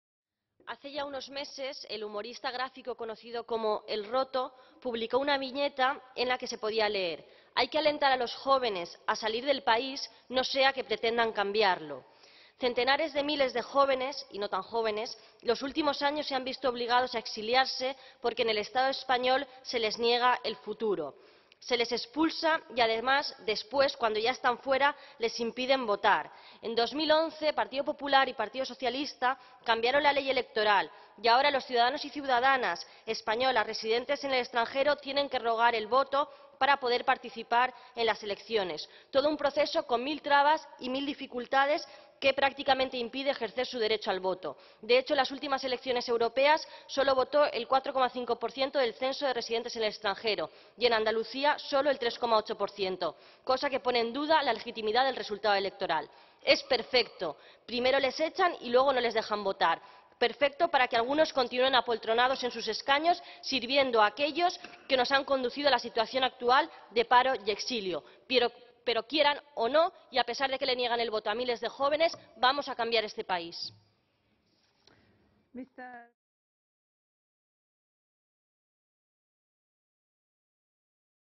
Albiol: "Vamos a cambiar el país aunque se impida a miles de personas votar desde el exterior"
Intervención en el Pleno del Parlamento Europeo de Marina Albiol, eurodiputada de Izquierda Unida, acerca de las trabas impuestas por el PP y el PSOE para poder votar desde el exterior por culpa del voto rogado.